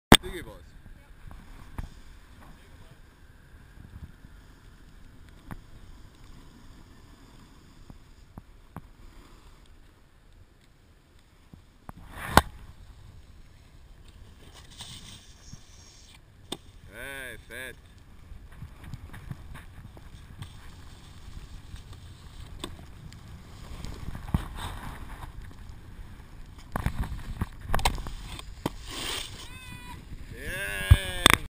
Pauli at Beginnerpark Stuhleck 12.22.2012
Pauli ripping the Boxline :-)